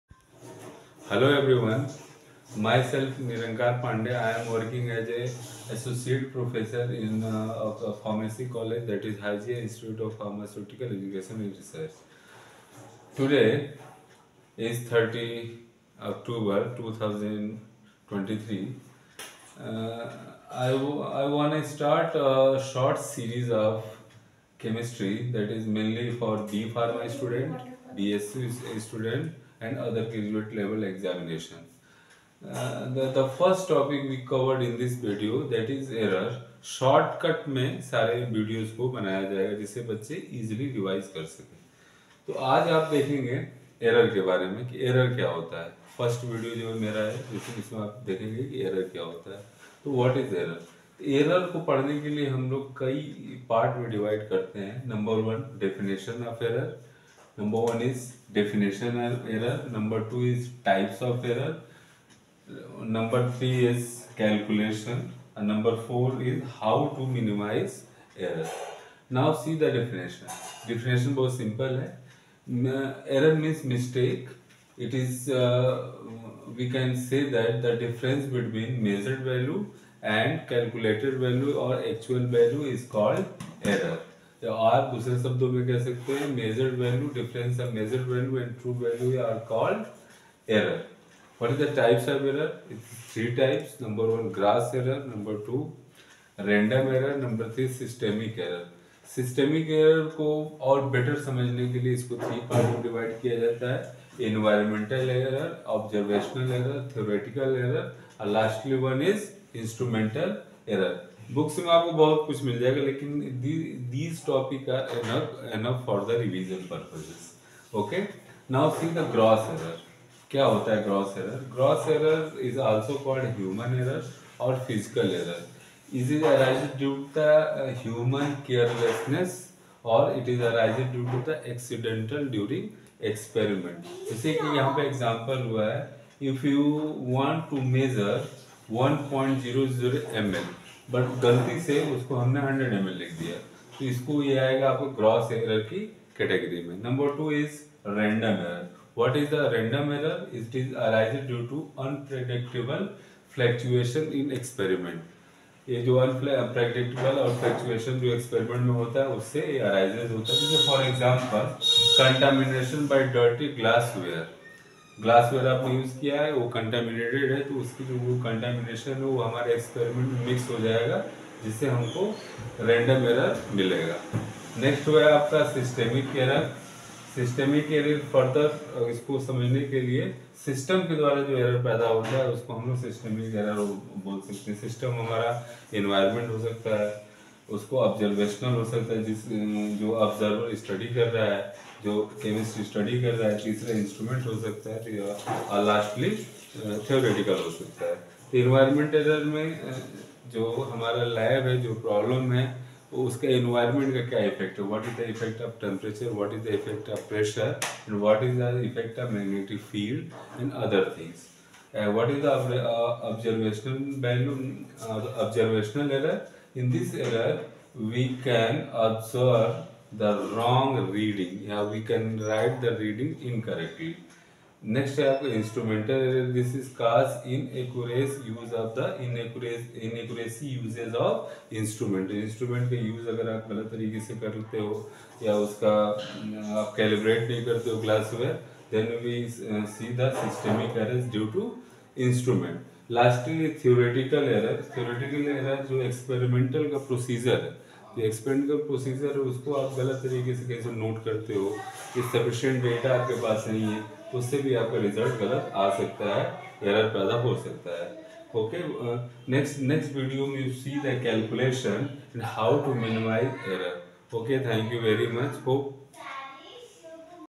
Error and types of Error
concept of error for BSc, B.pharm &class 11 students.
error  is the difference between the experimental and true values. Error=experimental value−true value. If the experimental value is less than the true value, the error is negative. If the experimental value is larger than the accepted value, the error is positive.